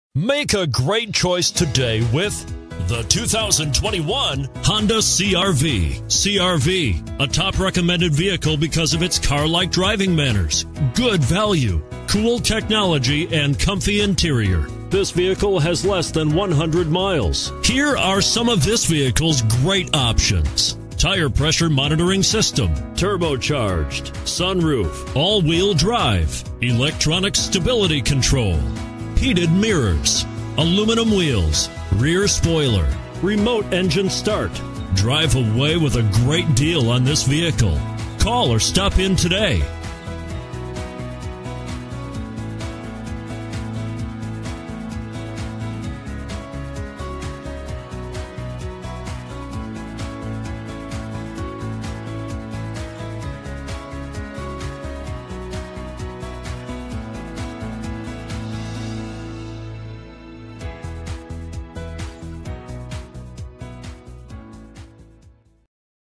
2021 Honda CR-V Nanuet NY HN211689
2021 Honda CR-V EX-L AWD

Modern Steel 2021 Honda CR-V EX-L AWD CVT 1.5L I4 DOHC 16VbrbrRecent Arrival! 27/32 City/Highway MPG